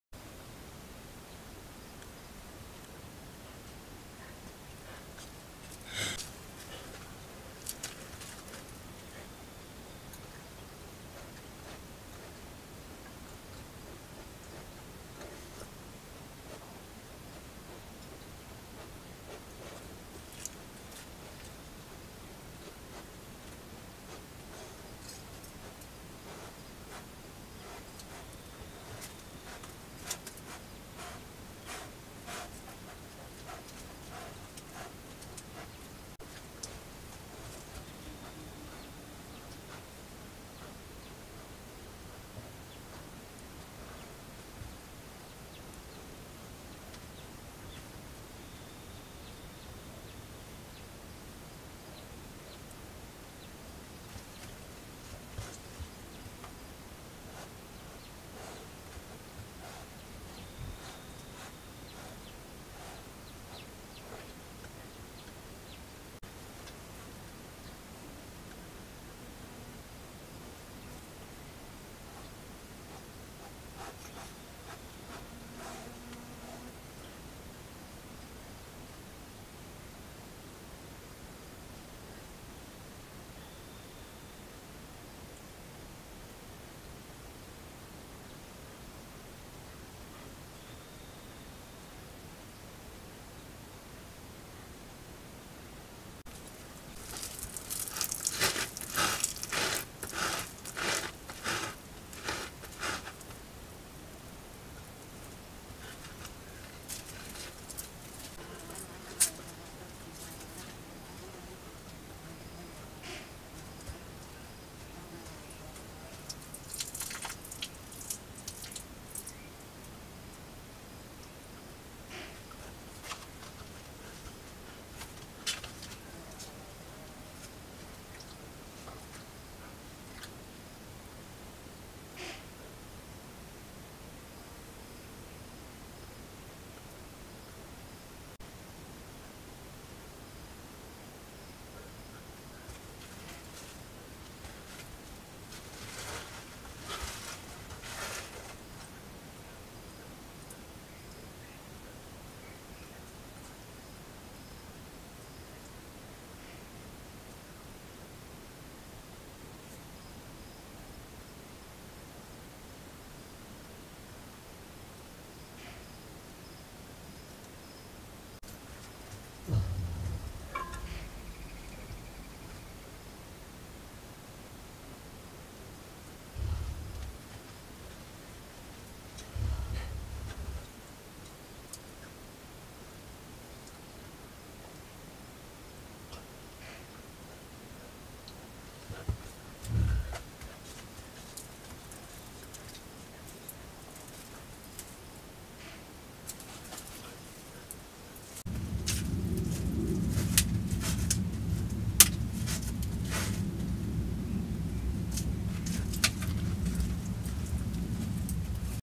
January 23, 2019
This is a Sequence of video of a female and her yearling offspring. She makes a few little scratches in some of the clips. Maybe this year I’ll get baby lions in the cave.